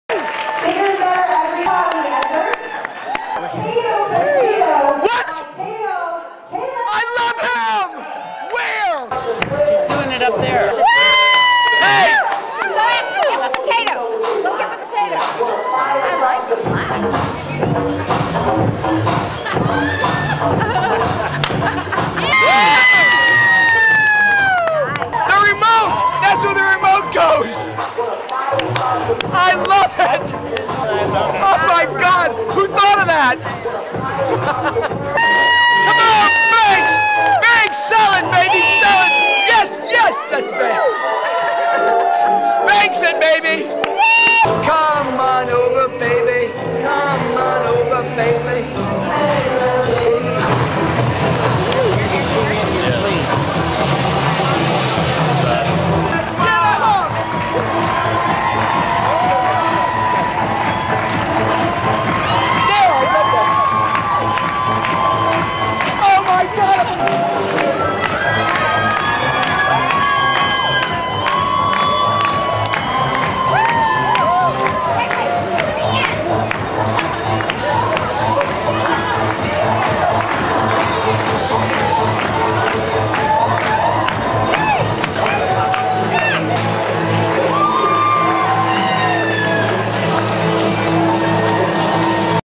Rhonda Shear and Kato Kaelin Tampa Fashion week
Kato joins Rhonda for Tampa fashion week with Kato Potatoe lounge wear...unleash your inner slacker